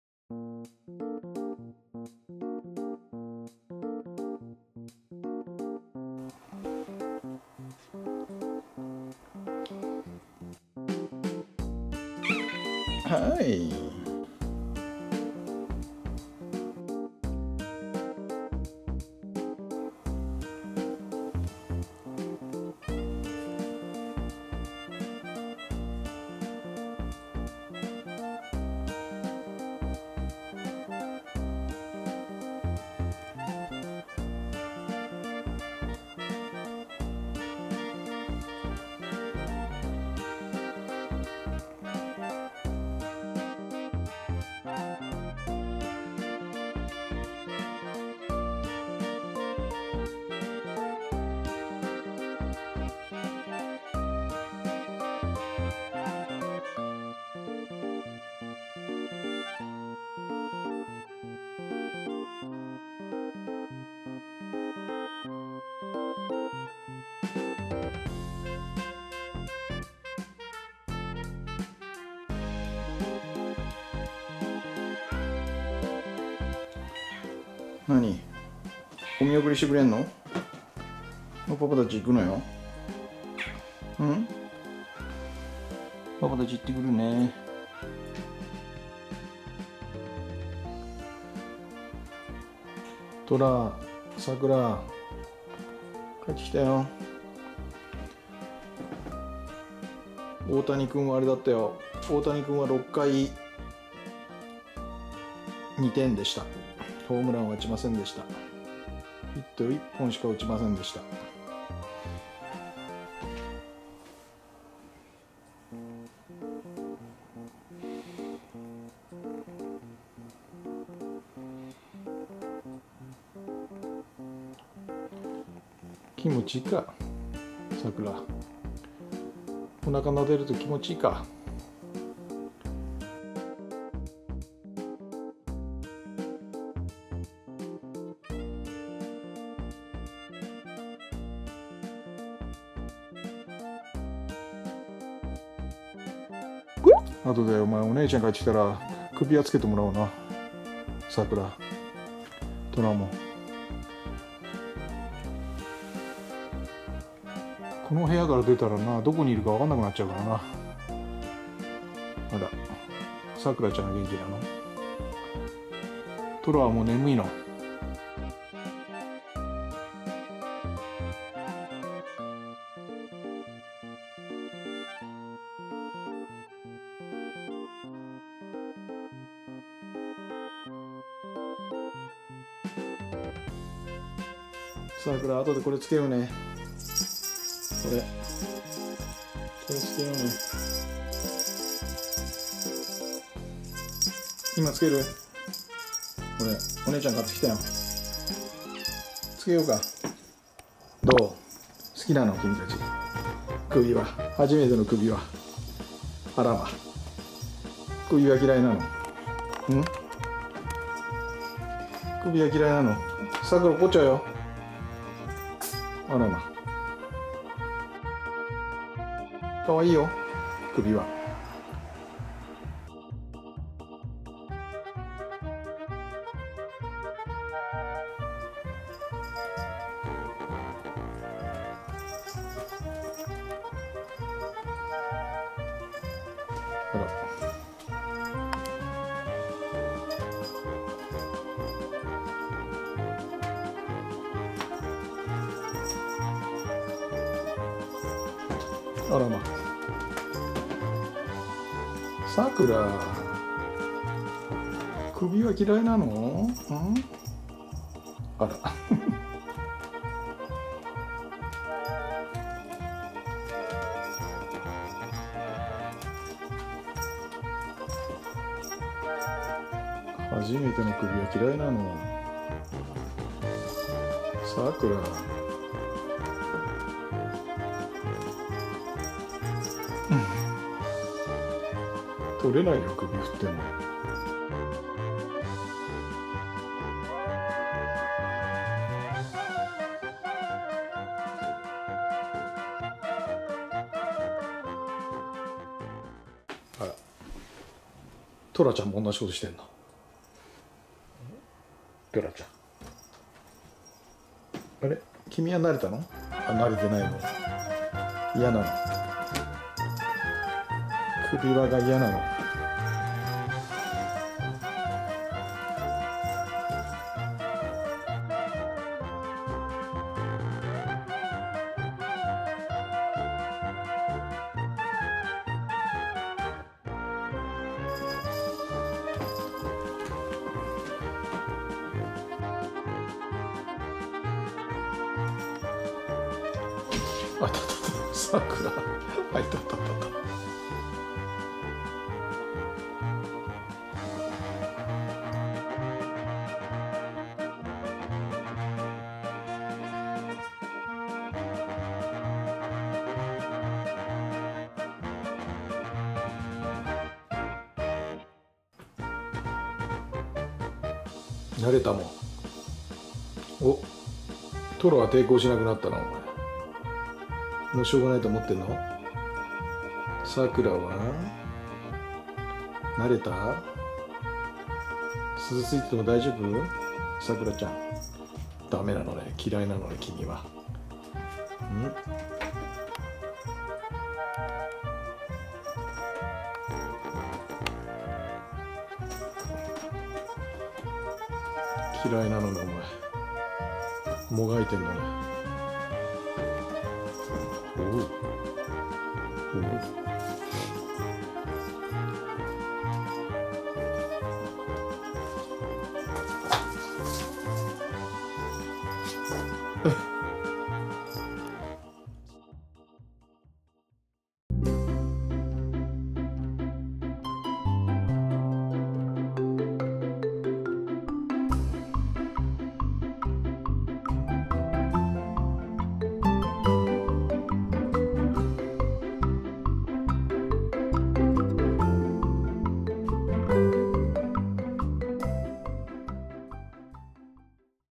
初めての首輪をつけた子猫
♣今日の動画
今日はトラとサクラ、初めての首輪です。